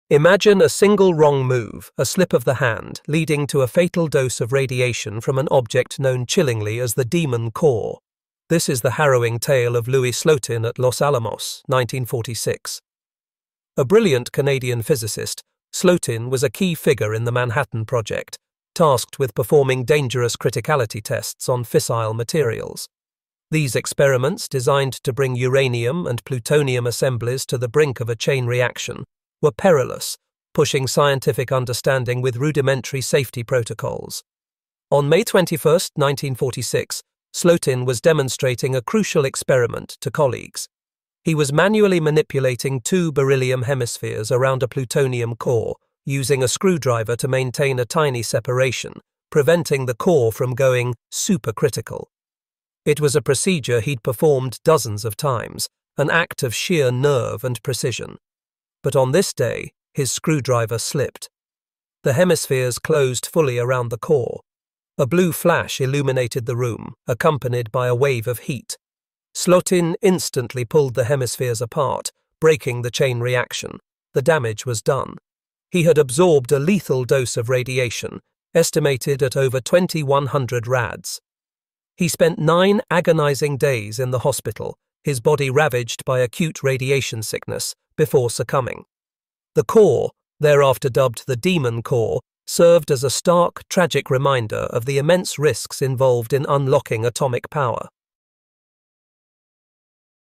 The Man Who Touched the "Demon Core" and Died (Louis Slotin) ☢️💀
It was an experiment nicknamed "tickling the dragon's tail," and on May 21, 1946, the dragon bit back.

In this video, we recount the terrifying true story of physicist Louis Slotin and the infamous Demon Core. At the Los Alamos laboratory, Slotin was performing a dangerous criticality test, using nothing but a flathead screwdriver to separate two beryllium hemispheres around a sphere of plutonium.

One slip of the hand caused the screwdriver to fall. The hemispheres snapped shut, and a blinding blue flash of Cherenkov radiation filled the room. Discover how Slotin heroically reacted to save his colleagues, sealing his own fate to a lethal dose of radiation that would kill him nine agonising days later.